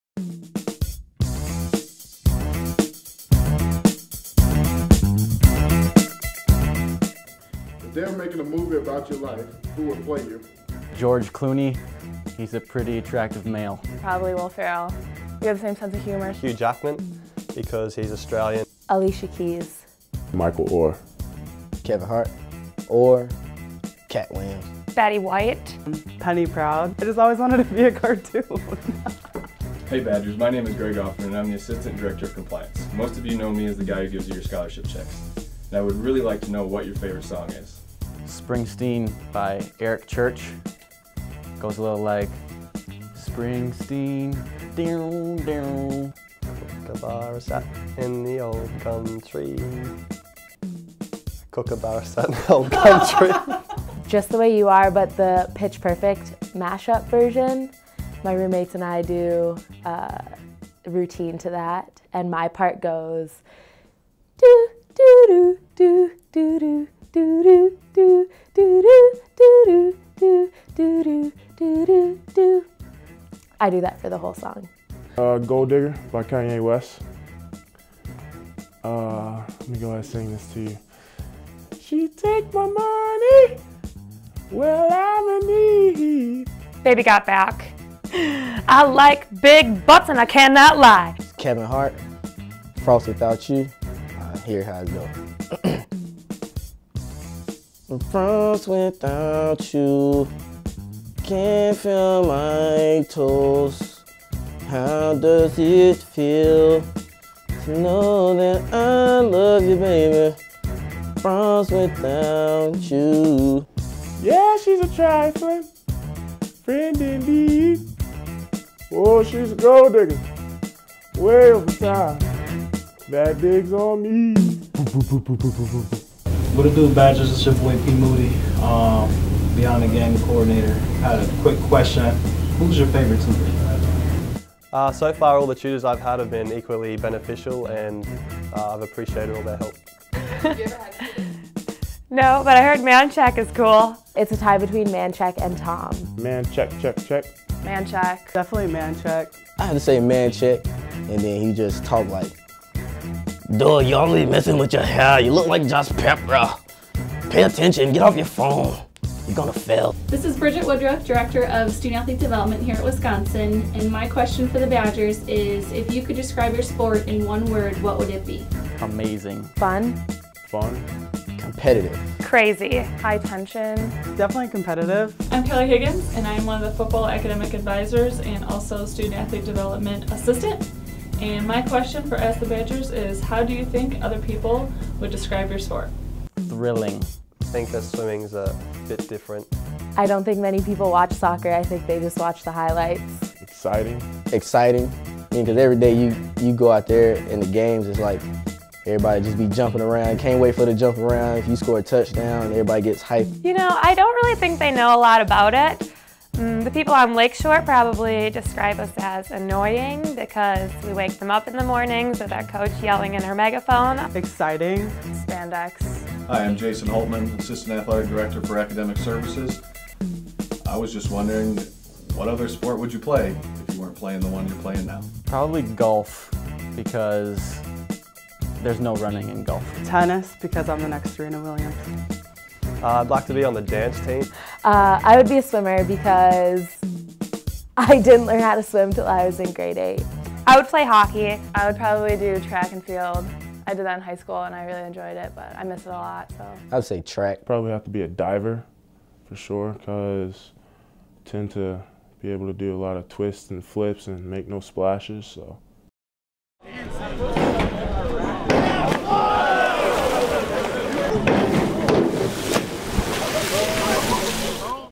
2013 Buckingham Awards: Ask The Badgers
As part of the entertainment for the 5th annual Buckingham's, Jade Davis of Women's Basketball put together an Ask The Badgers video segment featuring student-athletes from a variety of Wisconsin programs.